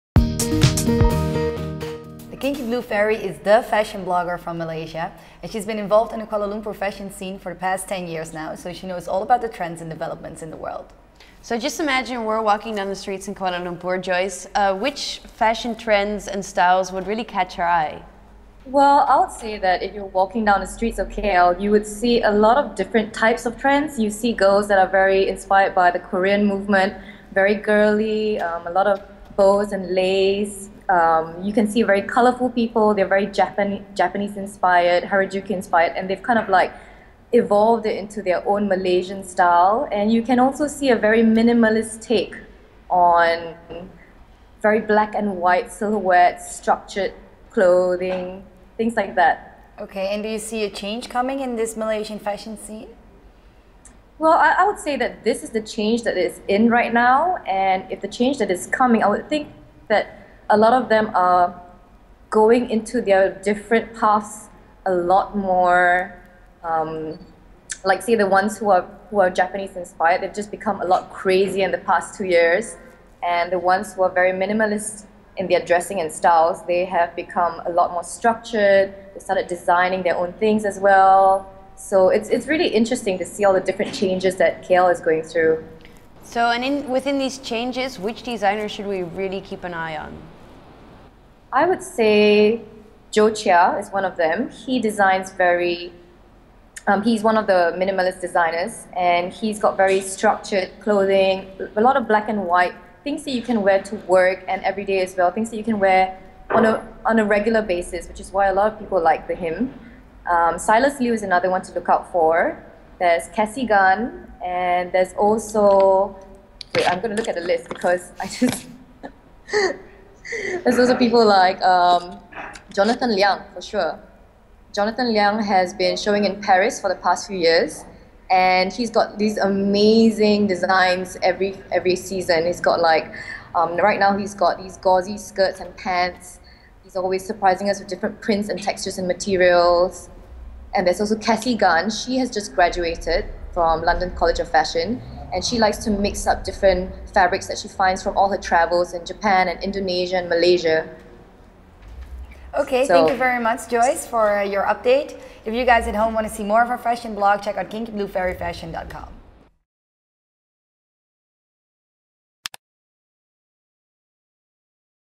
Why Your Wardrobe Needs A Malaysian Designer Piece
Fashion might not be the first thing that comes in mind when you think of Malaysia. However, they have raised a generation of quite some awesome designers that are conquering the world at the moment. In the video above the latest Malaysian trends and hottest designers are discussed.

Before we tell you something more about the designers, we would like to introduce you to our lovely contributor from Kuala Lumpur Joyce Wong. She is the one joining us for IntoCulture to talk about the Malaysian fashion scene. This pink haired fashionista started a blog in 2004 under the name 'The Kinky Blue Fairy’. The blog is very popular amongst Asian fashion enthusiasts and Joyce features IntoConnection to discuss the ins and outs of the wonderful world of prints and fabrics.

During the interview she mentions four Malaysian designers we should keep an eye on: Silas Liew, Jonathan Liang, Cassey Gan and Joe Chia, we’ll highlight the first two of them for you.

Silas Liew used to be in marine biology but then made an enormous career switch and started as a fashion designer. The Malaysian designer is famous for reconstructing traditional garments and combining aspects from national costumes of Asia and Africa. With his custom made collections contain wearable tribal costumes and his clothes are perfect to wear at special occasions and summer festivities. His Eid Collection is described as modern, bold, unique and wearable.

Designer Jonathan Liang is well known for his amazing designs and the broad mix of different materials. He draws his inspiration from sources like the Pink Panther, Shakespeare and the Burning Man event.
After only one season as a designer, Jonathan was invited by the London Fashion Week to feature in the  “A La Mode” showcase. This showcase displays a collection of the world’s most outstanding emerging and independent designers in front of press, buyers, industry experts and consumers. Next to England he also went to Paris: the dream of every designer.

If you want to know more about Joe Chia and his beautiful minimalistic designs, make sure you check out his blog filled with black and white. When you are more into Cassey Gan’s colorful fashion, you can find out more on her and her designs over here.